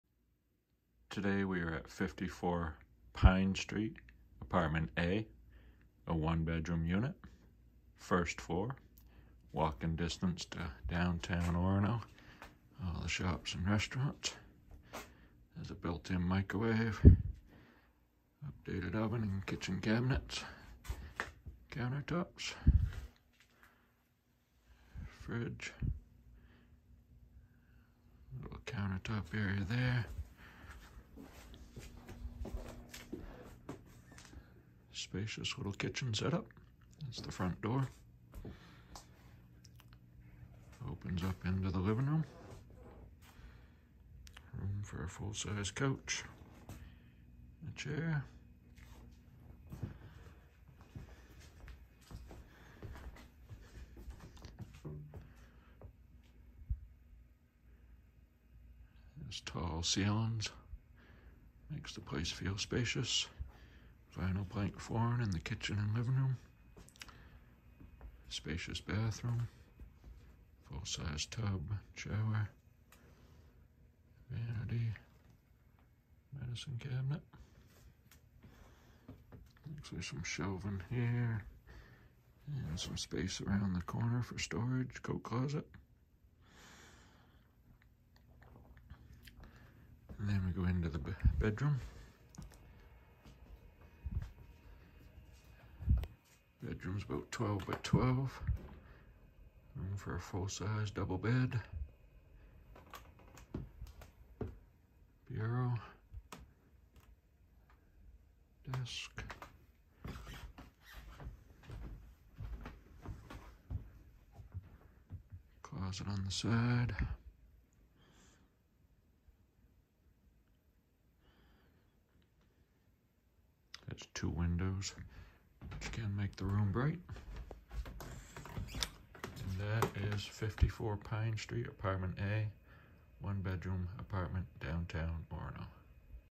54 Pine Street A, Orono, ME
One bed apartment in downtown Orono, Maine. Close to the University of Maine campus. 1st floor unit.